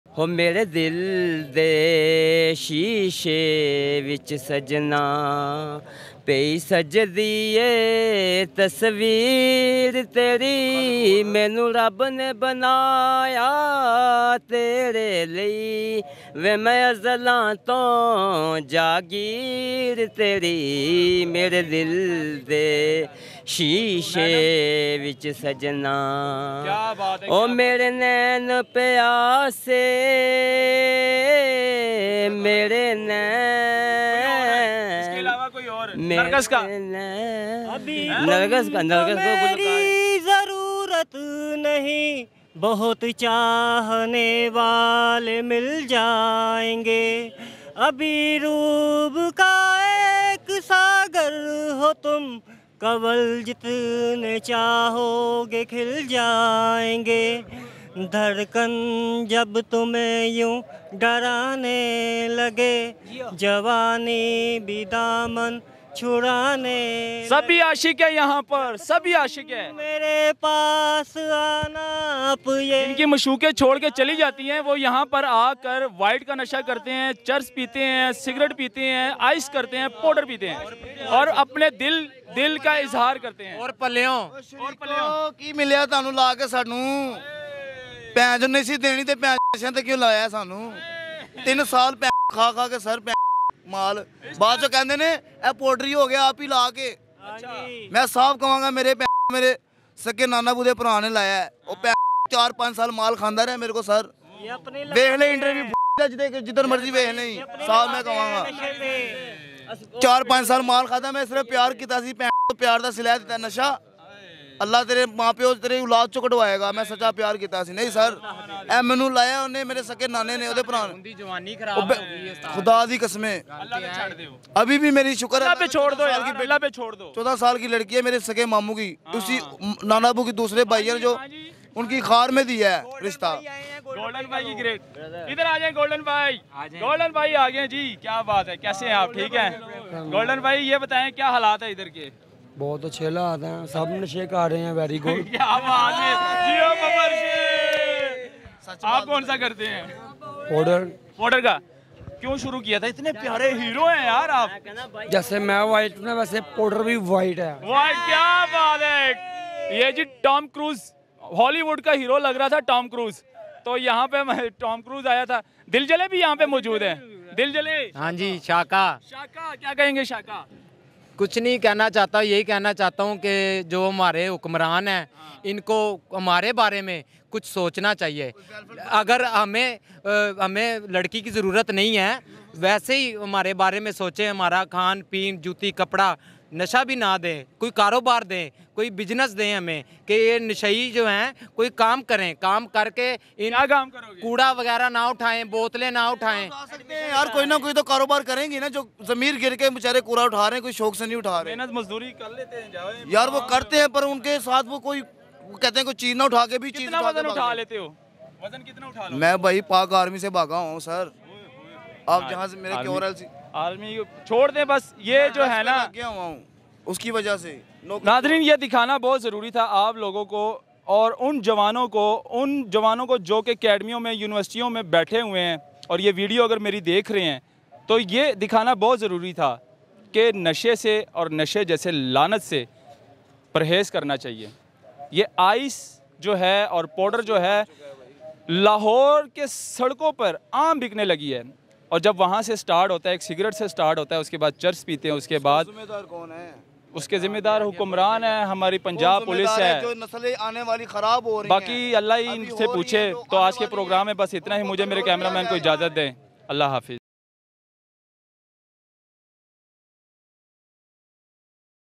20 Nashai ne Aesa Gana Gaya Anchor bhi Hairan Ho gya || Video Viral
Nashai ne Aesa Gana Gaya Anchor bhi Hairan Ho gya || Video Viral ||